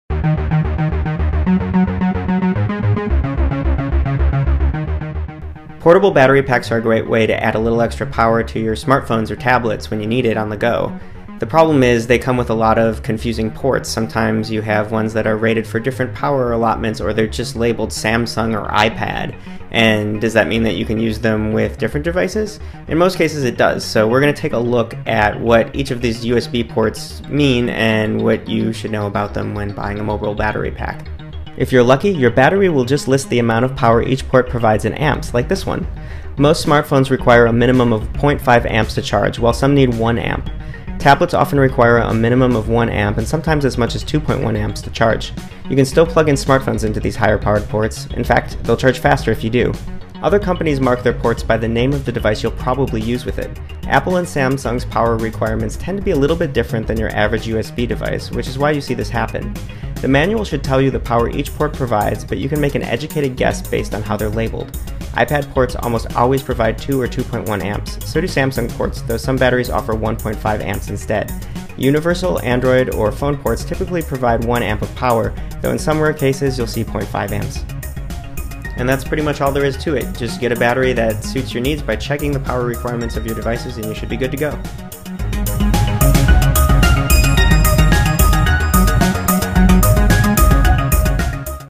Understand What Each Port Does on Your Mobile Battery Pack
Mobile battery packs can power your USB devices on the go, but they often come with ports that offer different amounts of power.  Poor labeling can sometimes add to the confusion, so here's what you need to know so you know which port to use for which device.